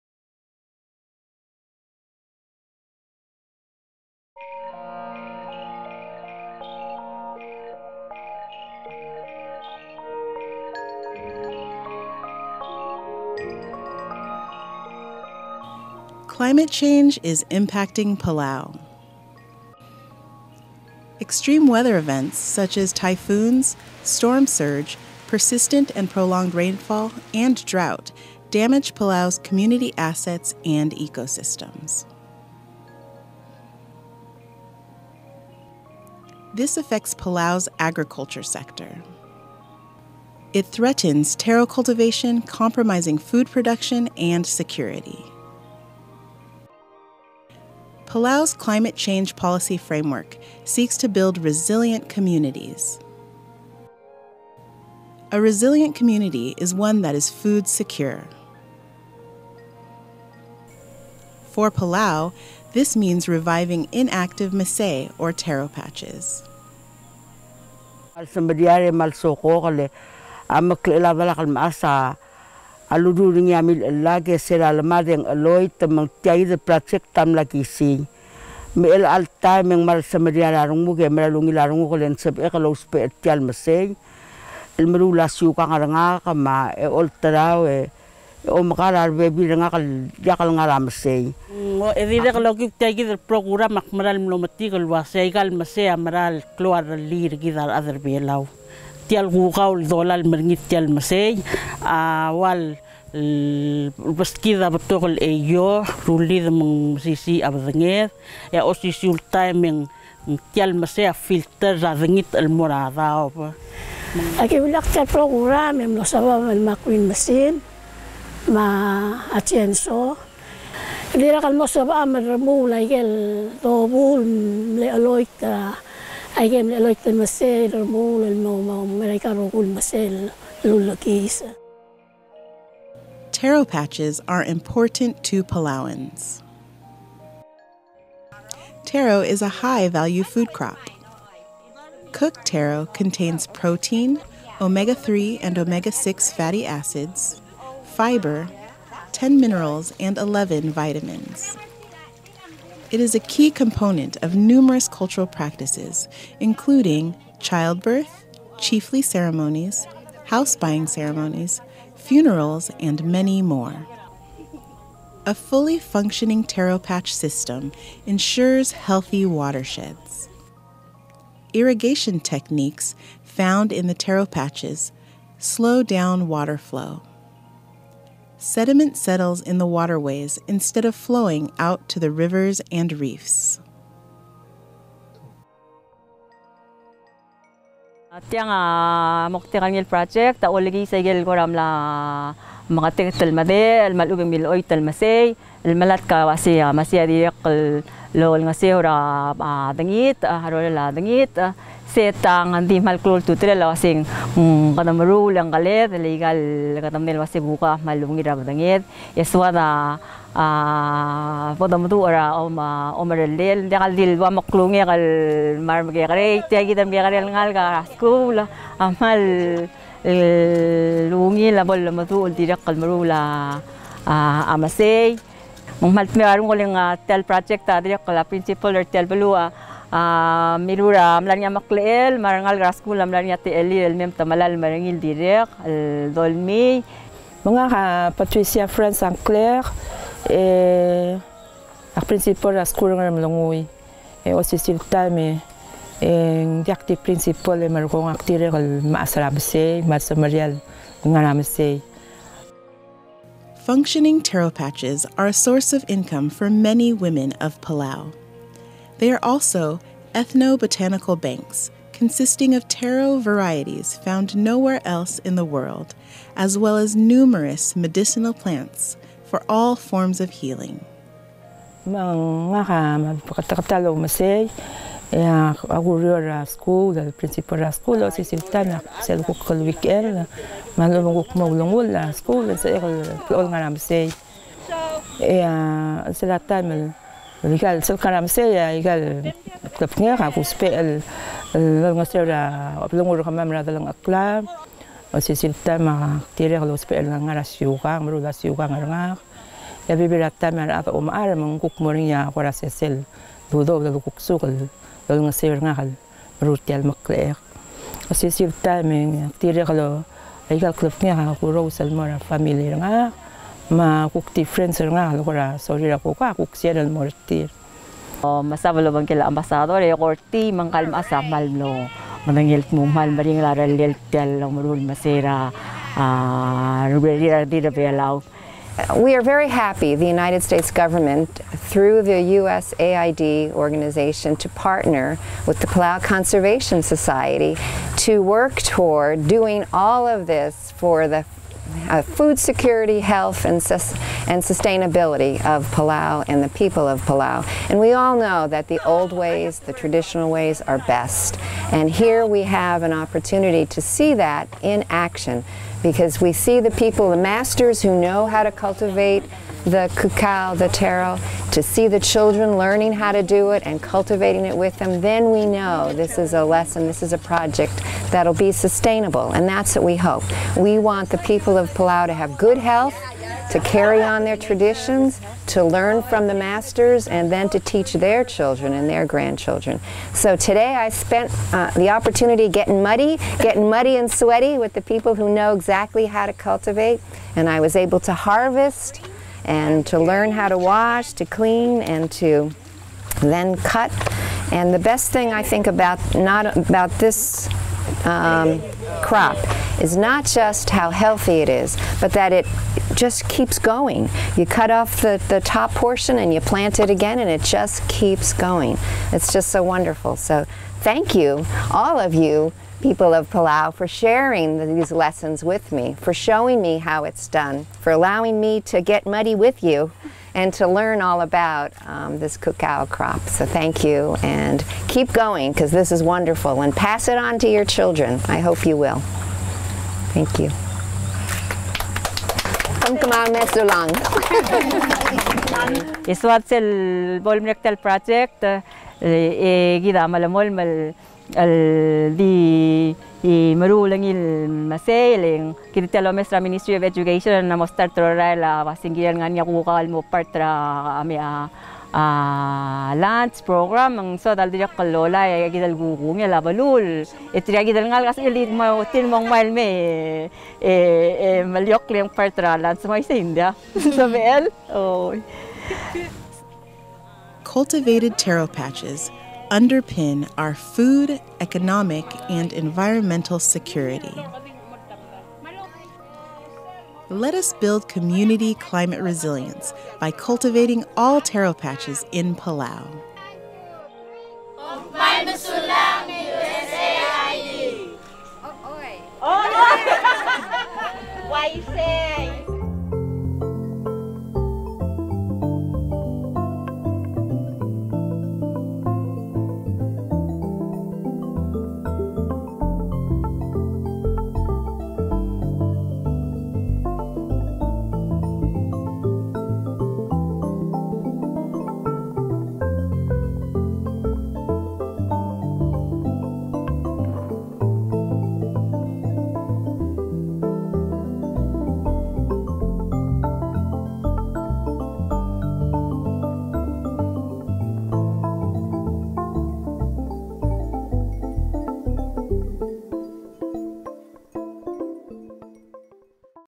Reviving traditional croplands to improve community climate change resilience
Modern life in the Republic of Palau has caused the traditional cultivation of Taro to become almost obsolete. This film documents those women who have held onto the traditional knowledge and how they are now teaching the next generation all the benefits of this long proven method of cultivation. They show how it will benefit them into the future and how it is possible to  maintaining both their modern lifestyle and that of their ancestors.